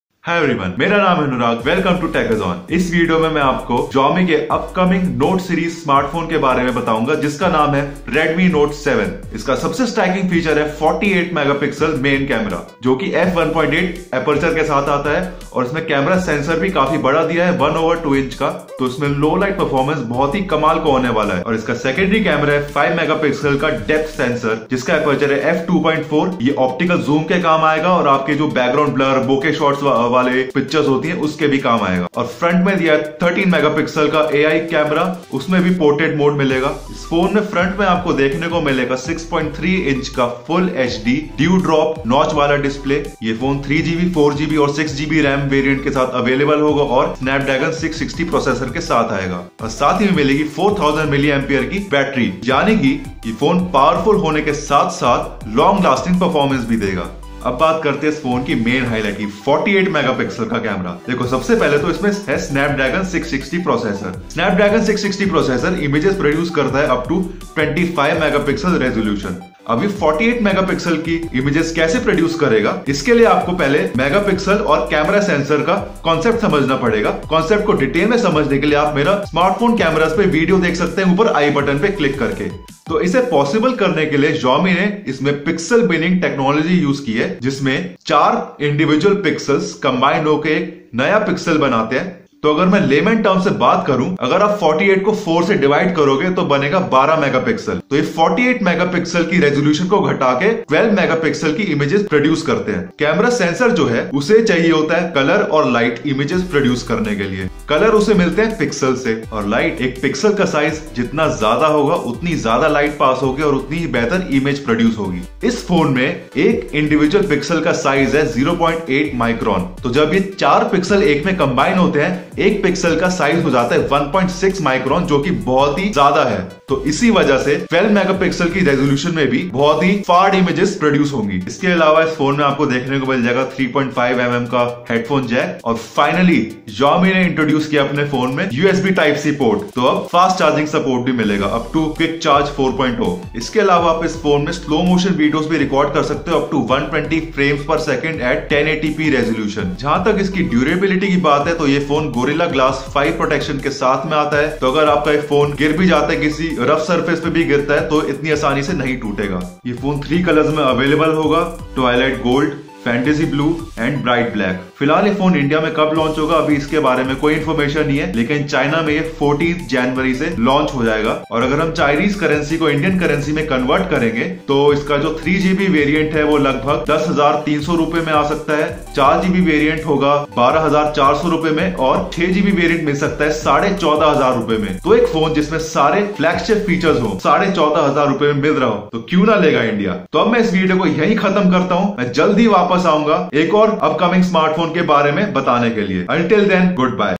Redmi Note 7 | Upcoming Smartphones of 2019 | 48 MP Camera Fake?
Xiaomi Redmi Note 7 launched in China with a massive 48-megapixel rear camera and a price tag that stunned everyone. Now, the smartphone is expected to launch in India soon and could easily outdo its predecessor with better specifications and an affordable price.

To recall, the Redmi Note 6 Pro launched in India with a starting price in India of Rs. 13,999 for the basic 4GB RAM version while the 6GB RAM version was priced at Rs. 15,999. We can expect the 3GB RAM version of the Redmi Note 7 to start at as low as Rs. 11,000 and the 6GB RAM model could be priced at around Rs. 16,000 in India. Well, these are just speculations, and we will have to wait for Xiaomi's announcement about the launch of the Redmi Note 7.
On paper, the 48-megapixel rear camera on the Redmi Note 7 looks marvellous but considering that the Snapdragon 660 processor is powering the phone only supports up to 25-megapixel leaves a lot of unanswered questions for a regular customer.

The answer to this mystery is the Samsung GM1 sensor which delivers higher quality 12-megapixel images. The new Samsung sensor can offer native 12-megapixel pictures but with the help quad pixels which combined can collect more light to offer up to 48-megapixel shots. Slightly technical but you will get the hang of it once you get your hands on the Redmi Note 7.
The Redmi Note 7 sports a dual camera set up at the back featuring a 48-megapixel sensor coupled with a 5-megapixel sensor. It comes with an f/1.8 aperture for better low-light shots. Xiaomi is also marketing the Redmi Note 7 as the phone with AI-enabled features including in selfies. There's also a 13-megapixel front camera onboard.

Running through specifications, the Redmi Note 7 features a 6.3-inch dewdrop notch full-HD+ (1080x2340 pixels) display with 19.5:9 aspect ratio. It comes with 409ppi pixel density. The handset packs massive 4000mAh battery with Quick Charge 4 support for fast charging. Unlike previous Redmi phones, the new Redmi Note 7 ships with a USB Type-C port. It will be available in Blue, Red, and Black colours with gradient look mimicking somewhat Honor 10-like rear. The Redmi Note 7 is built out of glass and looks very premium. It has been launched in 3GB RAM + 32GB storage, 4GB RAM + 64GB storage, and 6GB RAM + 64GB storage options.